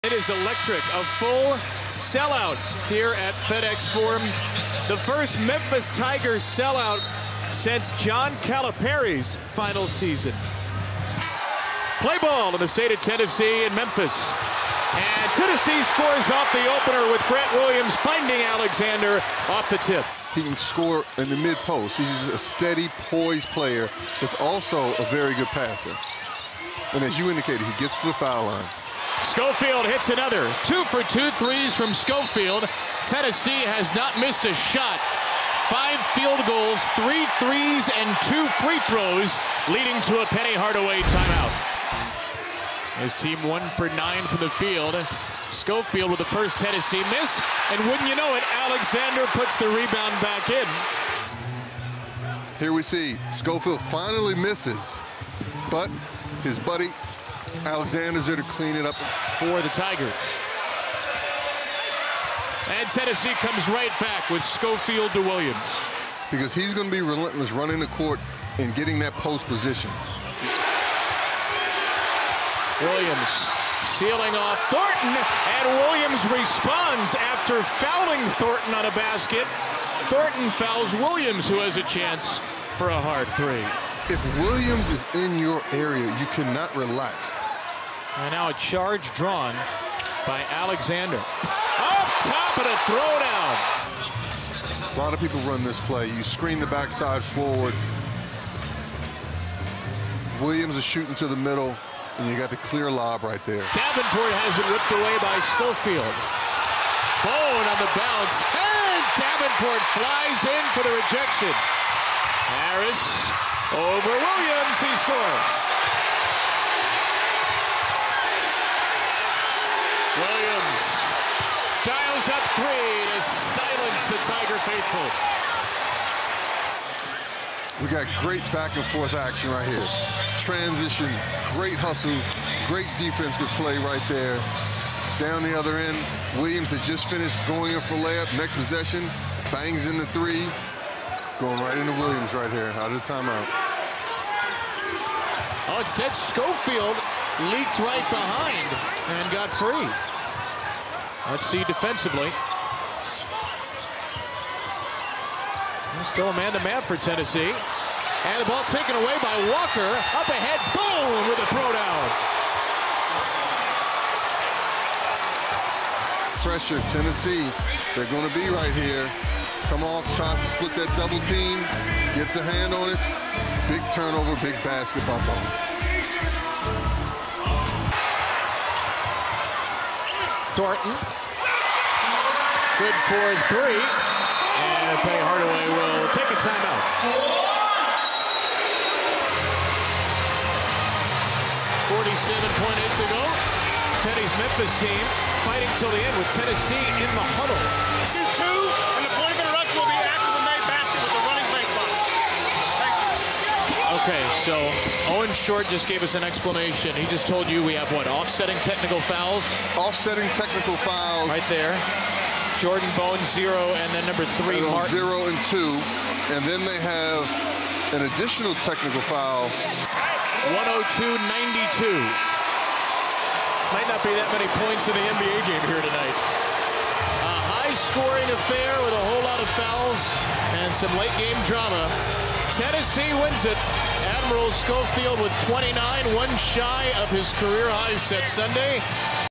Tennessee beats Memphis behind Admiral Schofield's 29 points | College Basketball Highlights
The Memphis Tigers get their first arena sellout since the John Calipari era, but the No. 3 Tennessee Volunteers spoil the party as Admiral Schofield scores 29 in the 102-92 victory.